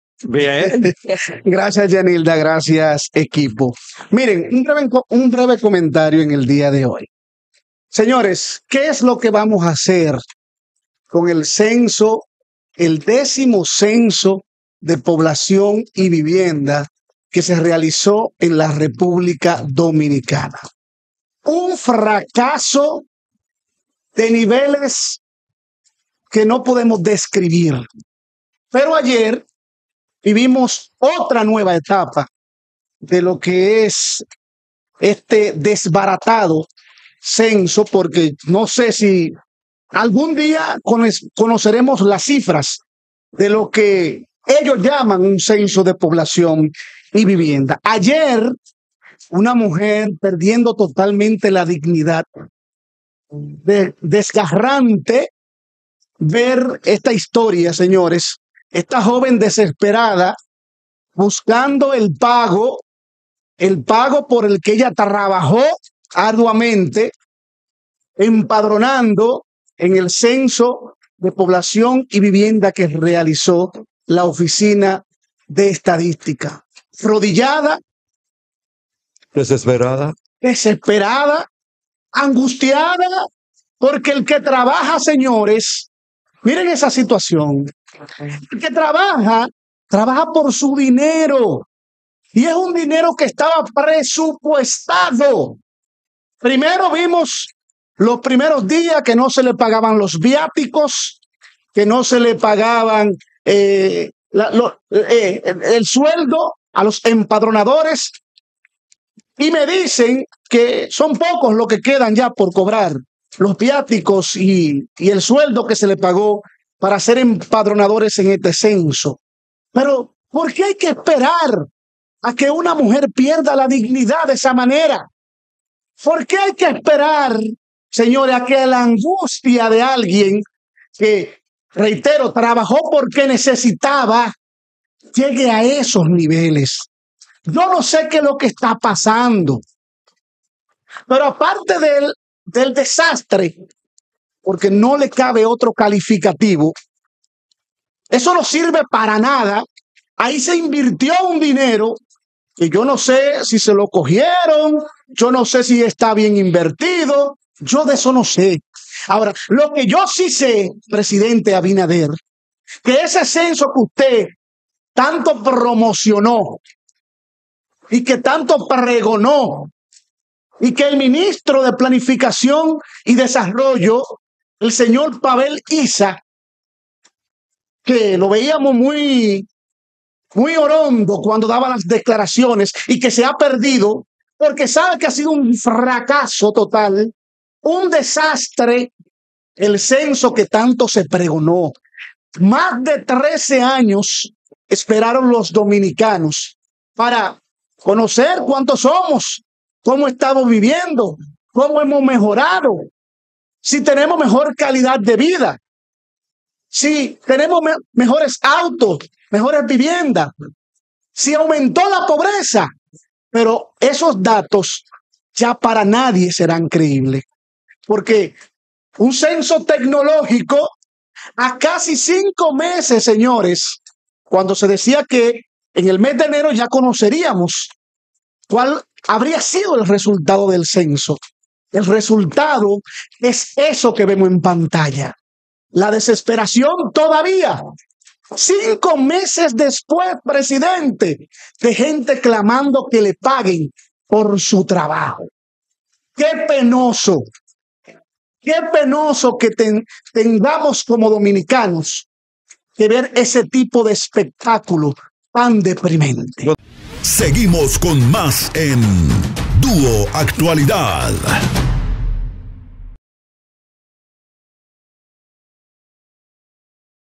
COMENTARIO - Mujer se arrodilla en la ONE reclamando pago por su trabajo en el CENSO
Plataforma creadora de contenido local con formato mundial compuesto por profesionales de la comunicación, siempre conectados con contenidos de actualidad.